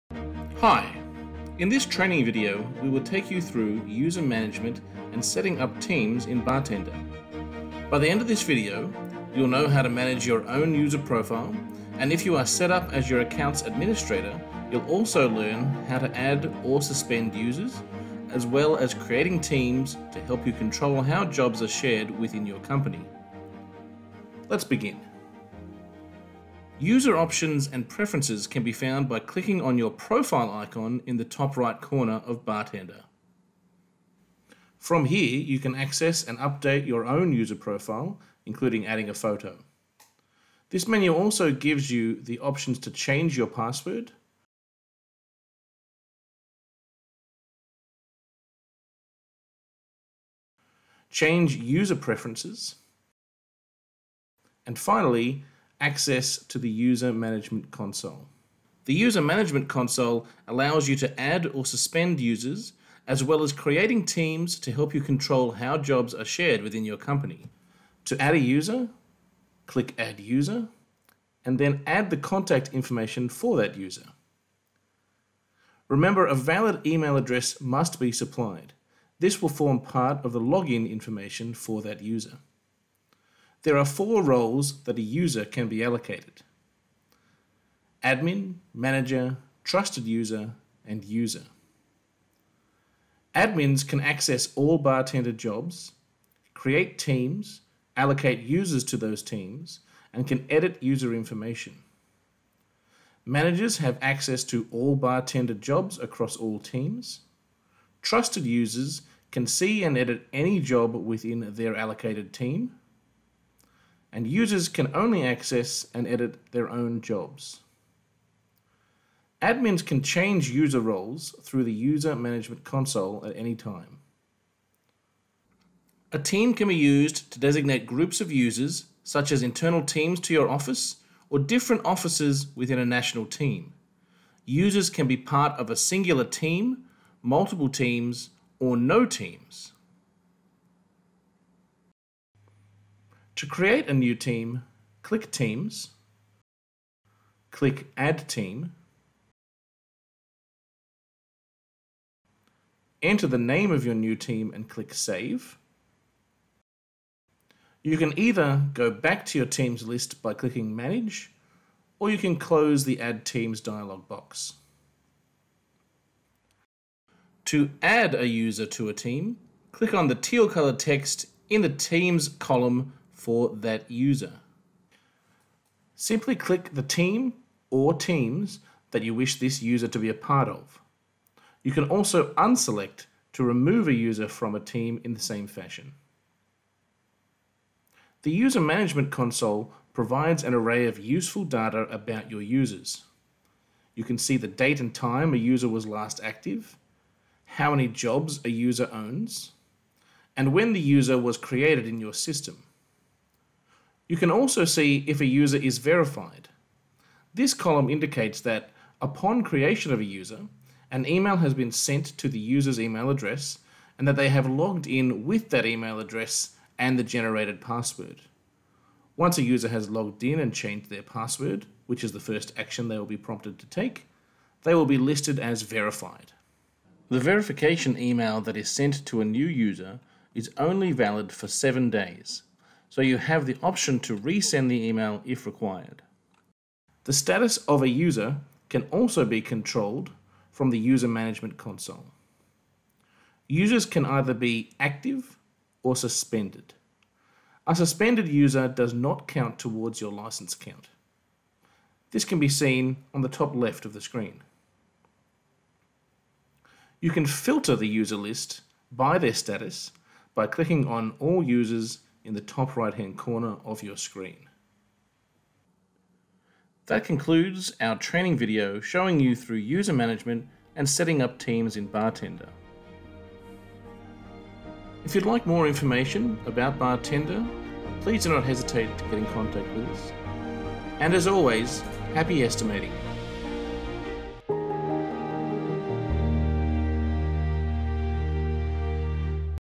How to manage users in Cubit Select
Learn how to manage users and teams in Cubit Select (formerly named Bartender) in this short tutorial. Developed by Buildsoft, Cubit Select is an innovative online platform designed to help you compare and select quotes from your subcontractors.